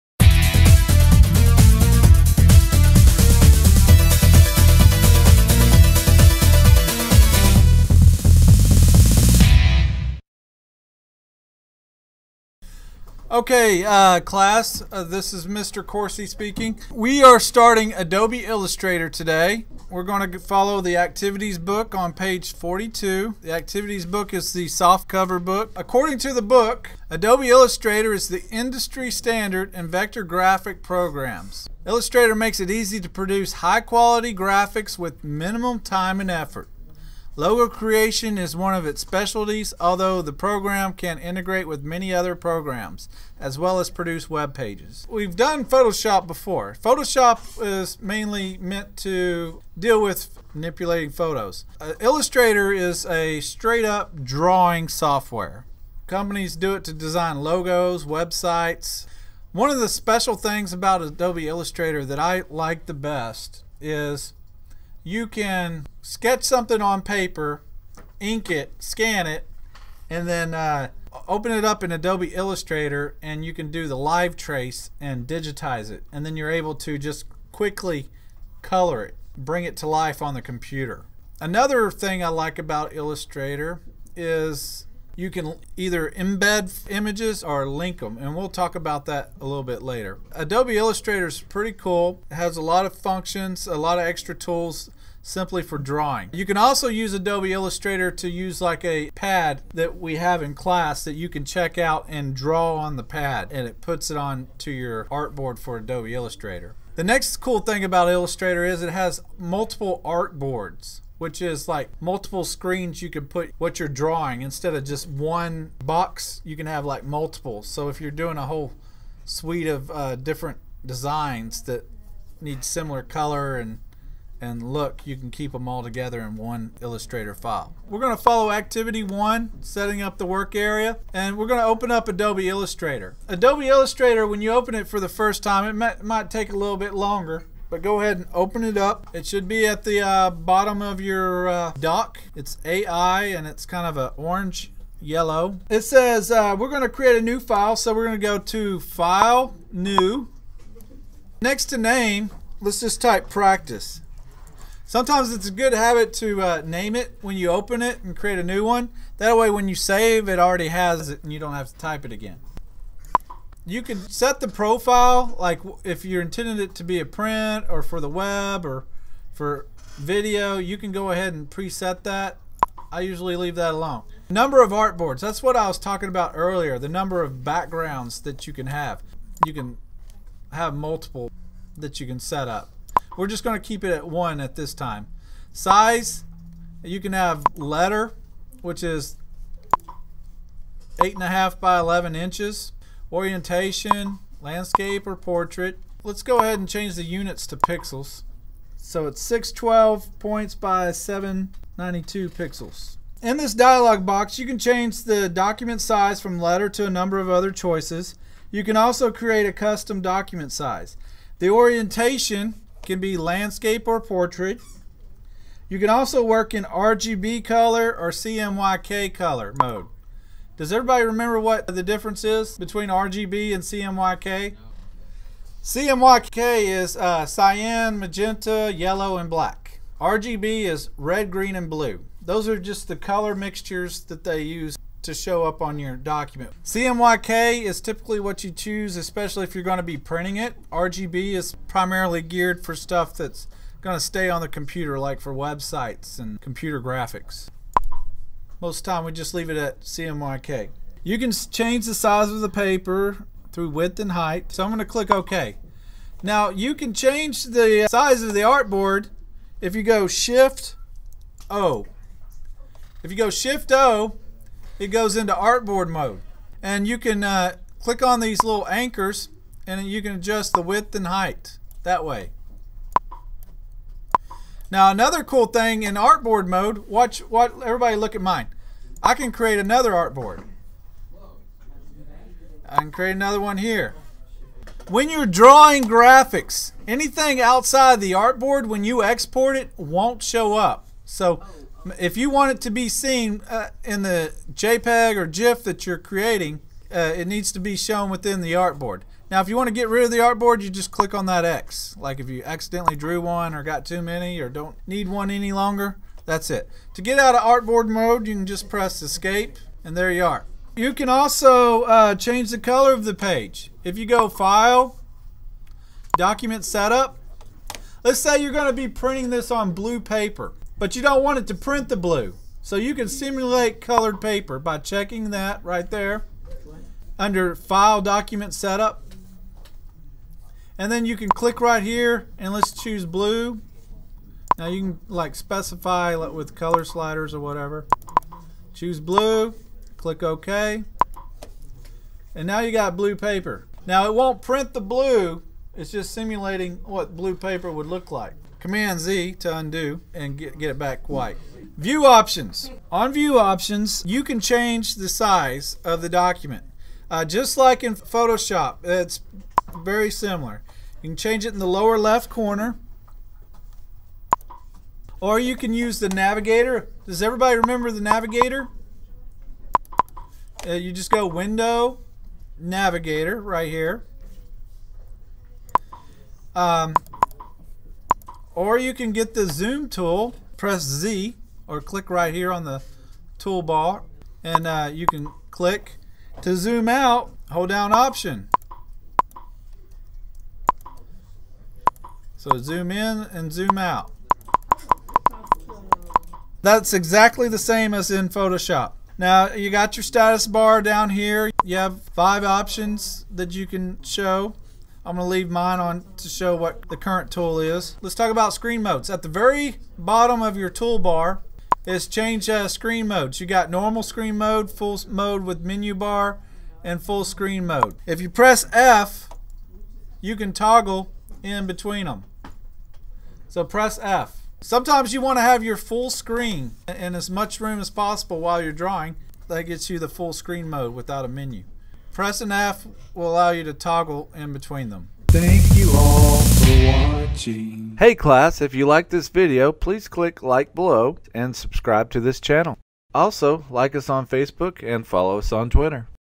Adobe Illustrator: L1 Work Area - Part1
L1:Work Area p.42

2:10 Open Illustrator
2:30 Open a New File - File/New name it practice
3:15 Change Units to Pixels, 612 px X 792 px
3:36 RGB vs CMYK
4:14 Click Ok
4:20 Shift + O goes into artboard mode to adjust the Artboard size and add dartboards
5:10 Press ESC to leave Artboard Mode
5:20 File/Documents/Setup You can change the background color to view how the image would look on colored paper. (This background color does not print)
5:50 View Options
6:10 Navigator
6:20 Press Z for the Zoom Tool. Option +click zooms out
6:40 Status Bar
6:50 Screen Modes. Press F to toggle between modes